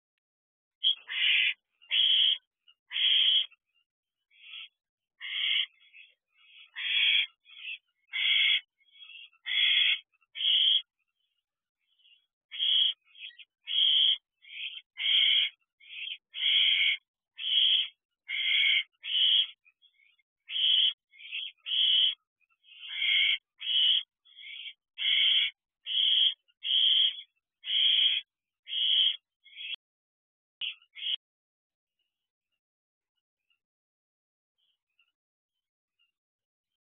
OwletPaulie Has a Large Rodent. Owl Box OSC - 6/14/23 - 10:39 pm
Owlets

Owl Boxes